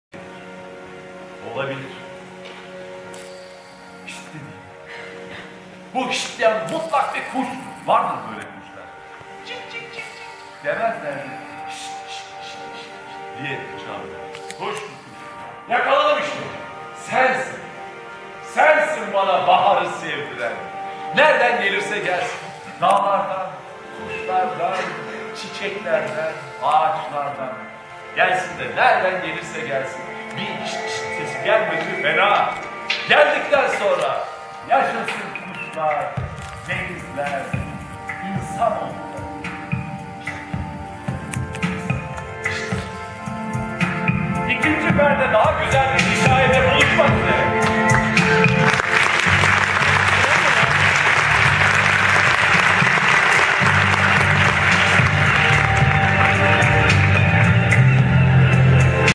Ankara Devlet Tiyatrosu Çaycuma'da(3)
Ankara Devlet Tiyatrosu tarafından “MERAKLISI İÇİN ÖYLE BİR HİKAYE” adlı oyun Çaycuma Kültür ve Sanat Merkezi’nde sahnelendi.14.01.2015